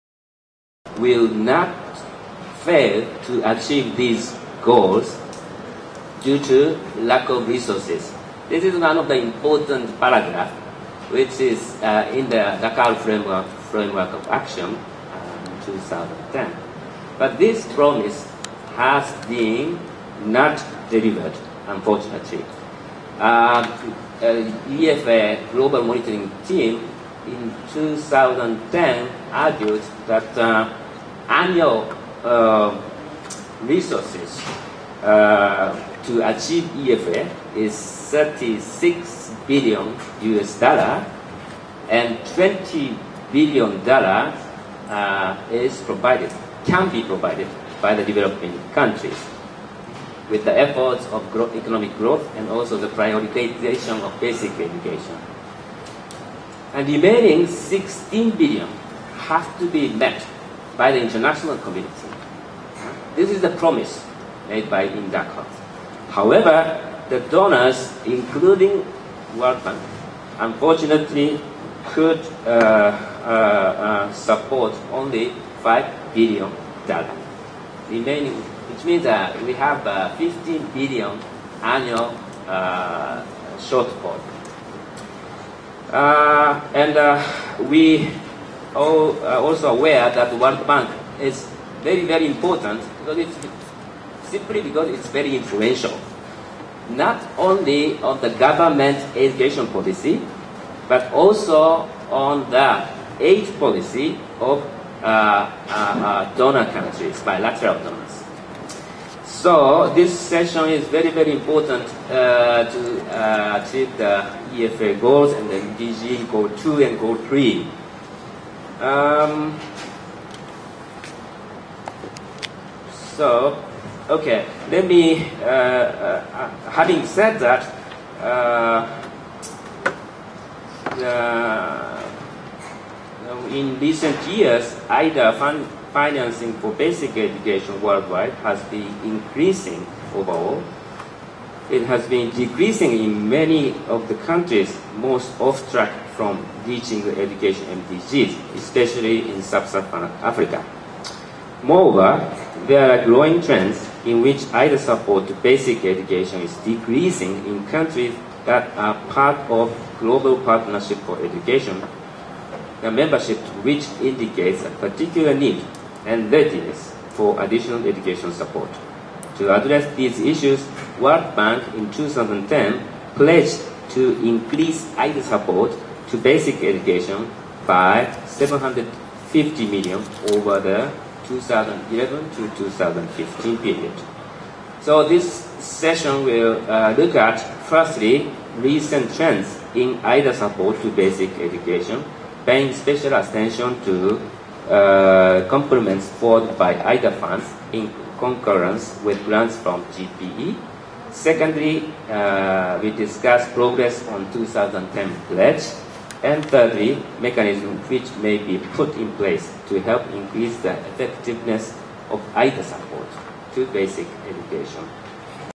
Takafumi Miyake - World Bank Annual Meetings, Tokyo 2012: Education Policy Session
Takafumi Miyake, Secretary-General of the Japan NGO Network for Education, speaking at a session entitled "Basic Education and the World Bank's 2010 Pledge" at the Civil Society Policy Forum of the World Bank Annual Meetings in Tokyo on October 10, 2012.